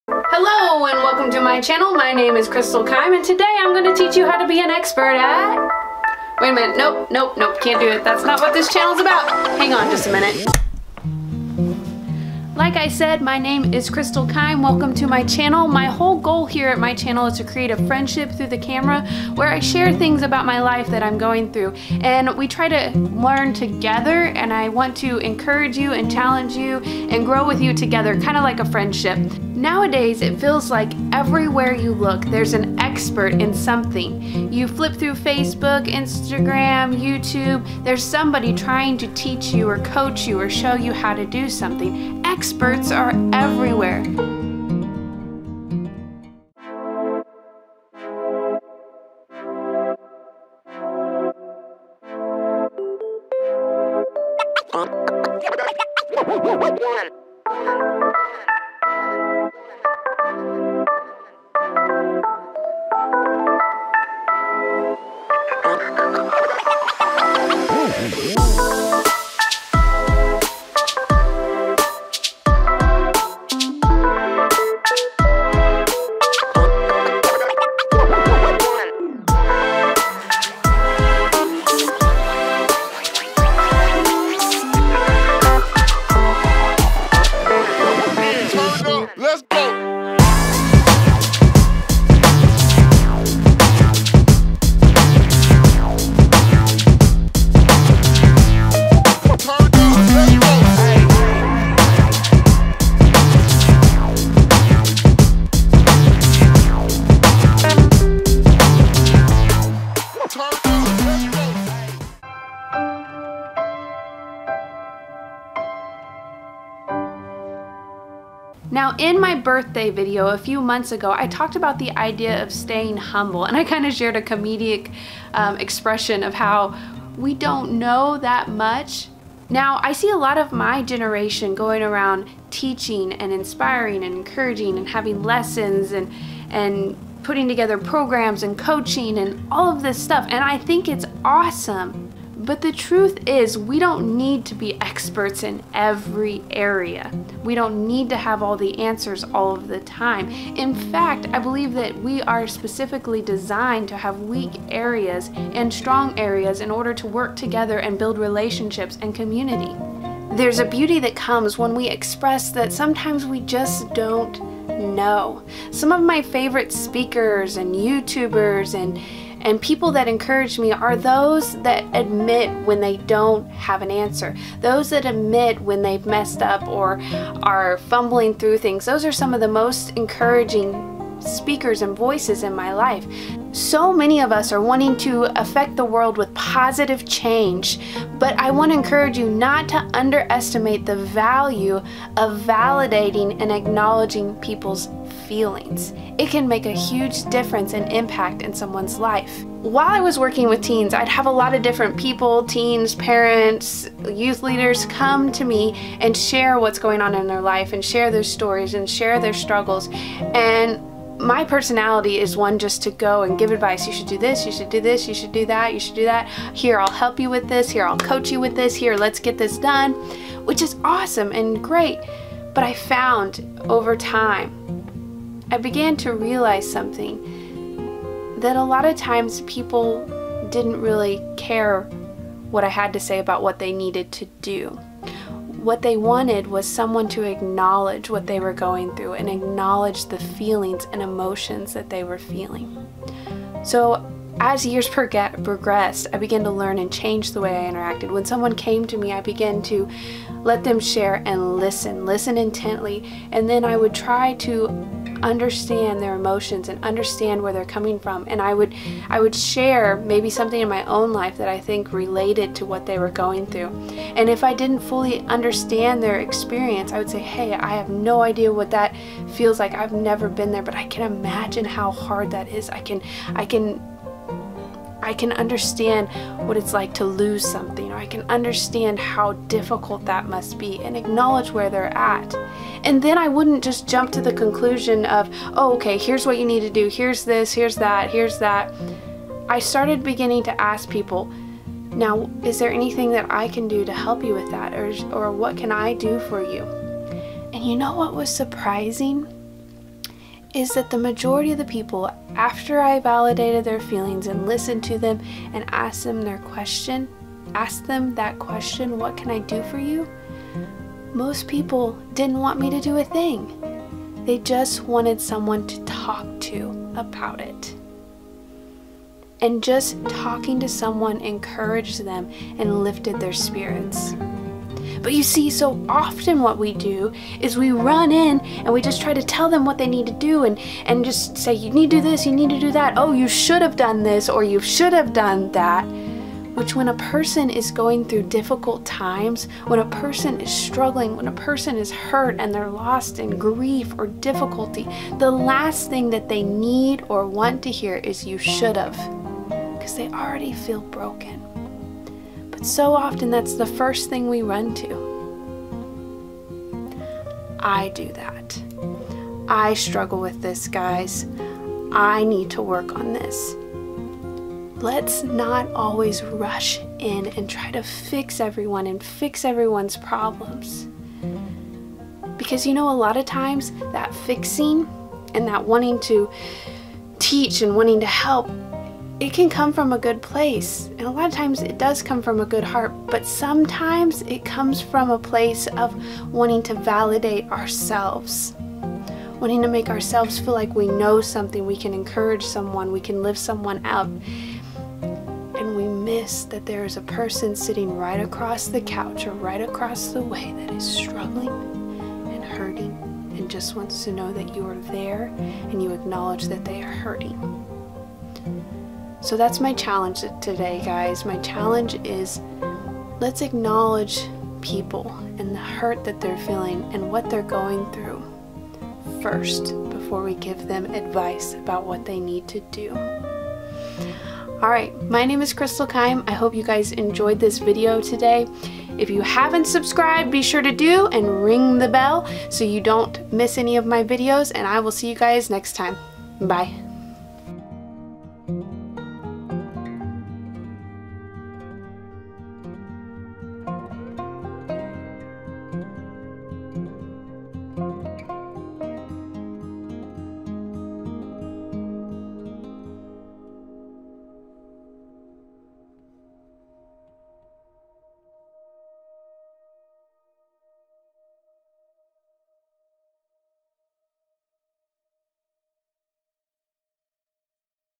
Validate Other's Feelings
It is so easy to want to always advise people, however most people shut down and don't receive any advice that we may have to give unless we first validate their emotions and feelings. People want to feel understood and heard. They want to know that people are there and understand what they are going through. In this video I talk about how I am learning and growing in the area of validating others.